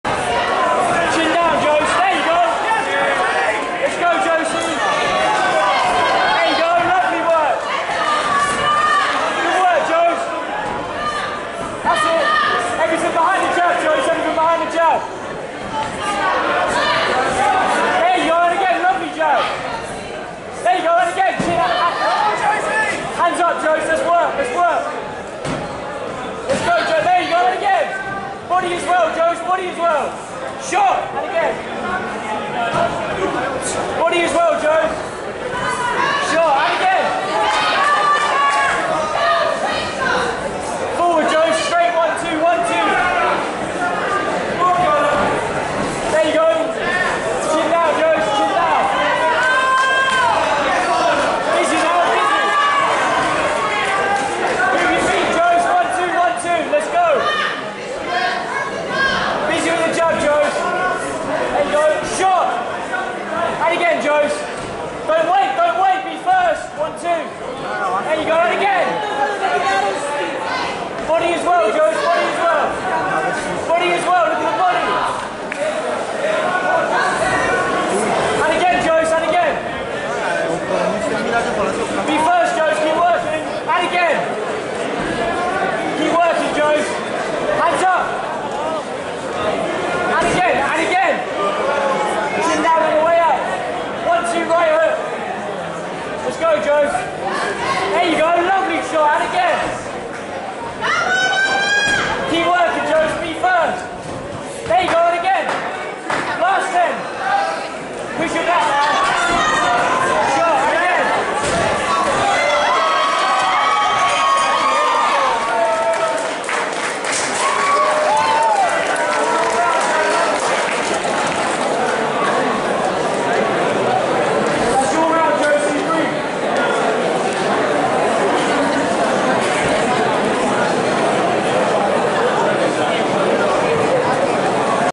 MAH03863 - BRISTOL UNIVERSITY BOXING CLUB/DOWNEND ABC HOME TOURNAMENT (UBSU ANSON ROOM) 02/03/2024
University of Bristol Boxing Club/Downend ABC Home Tournament:
The Anson Room, University of Bristol Students' Union, 105 Queen's Road, Clifton, Bristol  BS8  1LN:
Saturday, 2nd March, 2024: 21:30 Hours:
Bout No. Ten of Sixteen:  Female Senior Bantamweight Novice Contest:  3 x 2 Minutes:  Round No. I of III:
Anna de Boer (Bristol University/Downend ABC) (Red) WOP (Majority) versus  Josie McGarrigle (Imperial College, London ABC) (Blue).